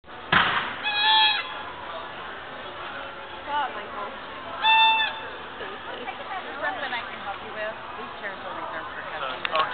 Squeeky Chair At Macy's
Messing around at Macy's..

"Is there something I can help you with?  These chairs are reserves for customers..."

she was interrogating me for a long time..haha.  she thought i was really making those noises and causing a rucus.

PS: that's my mom in the background saying "what's that noise??" lol